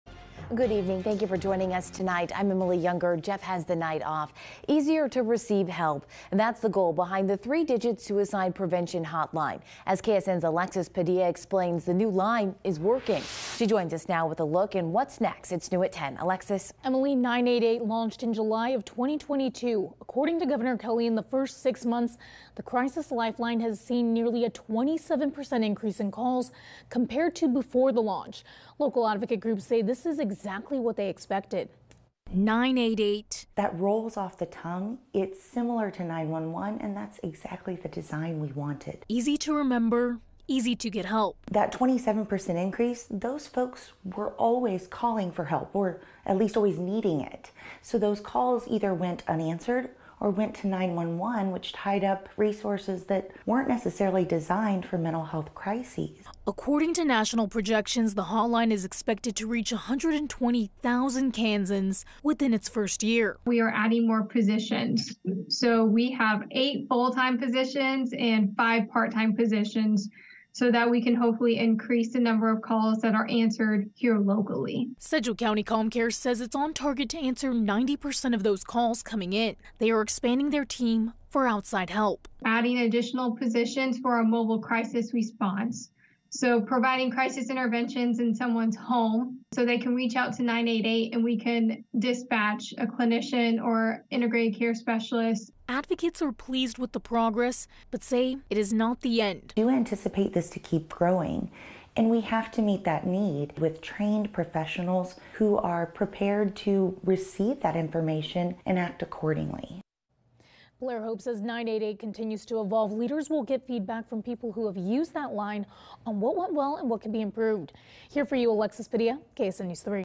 New 988 crisis line sees 27% increase in calls in Kansas
he 988 Suicide and Crisis Lifeline has seen a 27% increase in call volume since it first went online six months ago in July of 2022